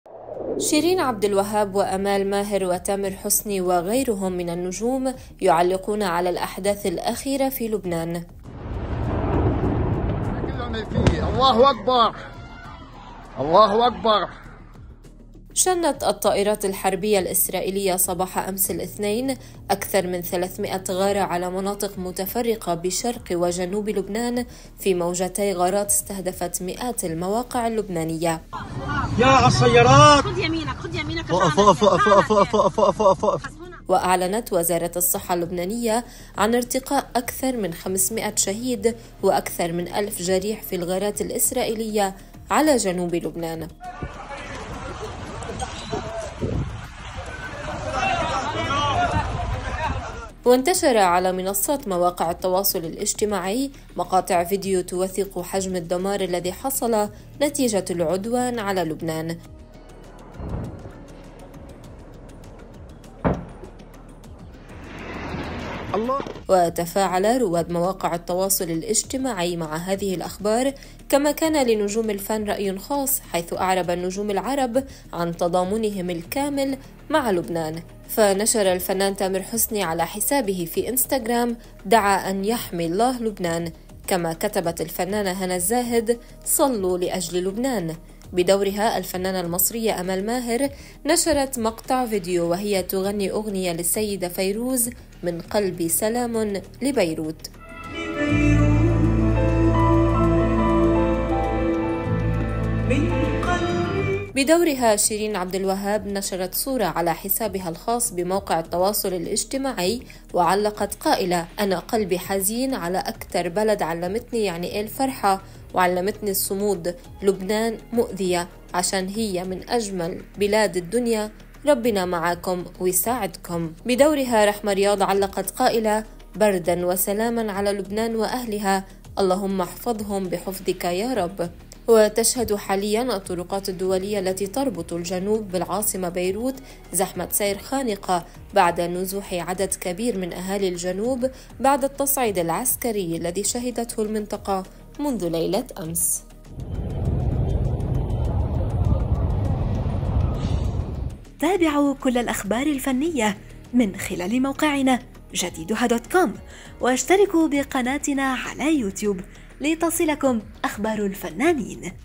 شيرين عبد الوهاب وآمال ماهر وتامر حسني وغيرهم من النجوم يعلقون على الاحداث الاخيرة في لبنان
شنت الطائرات الحربية الإسرائيلية صباح امس الاثنين أكثر من 300 غارة على مناطق متفرقة بشرق وجنوب لبنان في موجتي غارات استهدفت مئات المواقع اللبنانية.

:يمكنكم متابعة آخر الأخبار الفتية عبر موقع جديدها